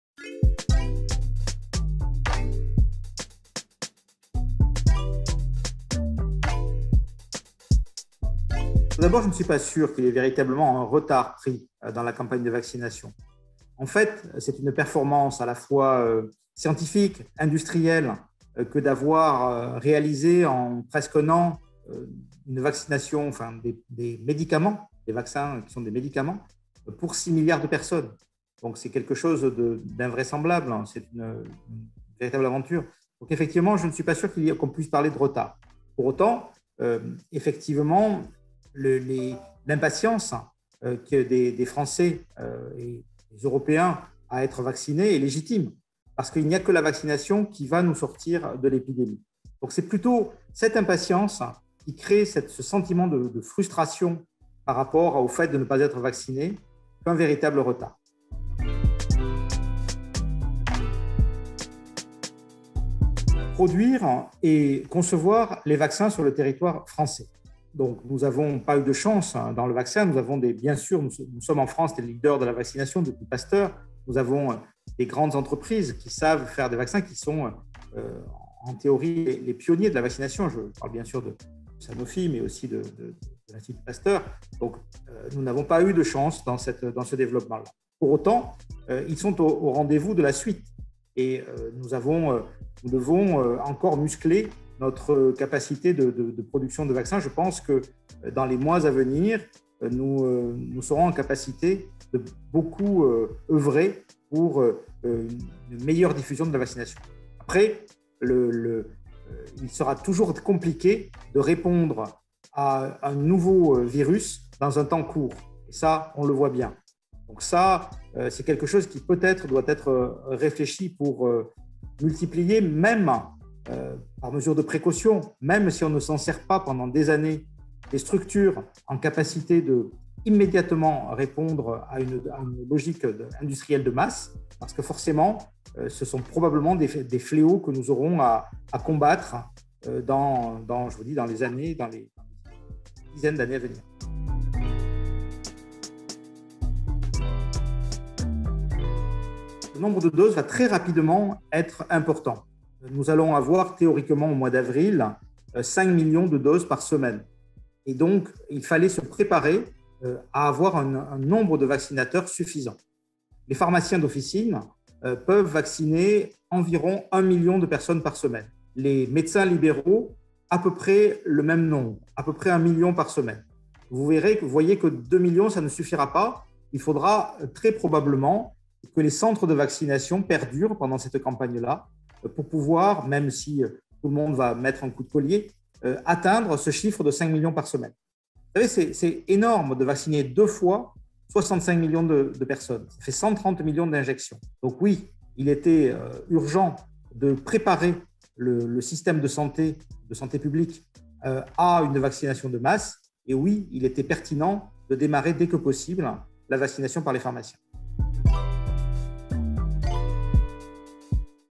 Philippe Besset (FSPF) - vaccination covid19 - CESE
Questions à Philippe Besset, Président de la Fédération des Syndicats Pharmaceutiques de France (FSPF), auditionné par la commission temporaire "vaccination" du CESE dans le cadre de la saisine gouvernementale "vaccination".
Le CESE a été saisi en décembre 2020 par le Premier ministre au sujet de l'accompagnement de la stratégie vaccinale contre la Covid-19.
Participez à la consultation en ligne sur vaccinationcovid.lecese.fr